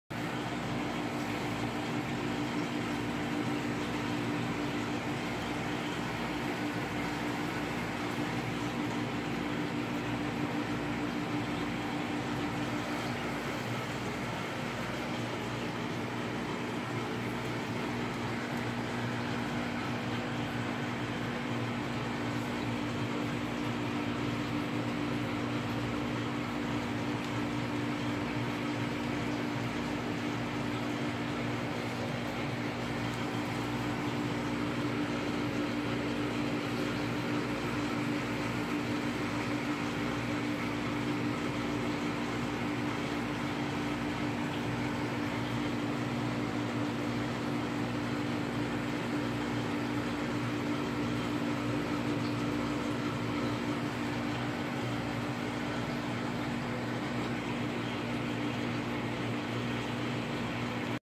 Calico Amatitlania nigrofasciata
Nice group of our Calico convicts breeders  at PR EAST COAST CICHLIDS-PA®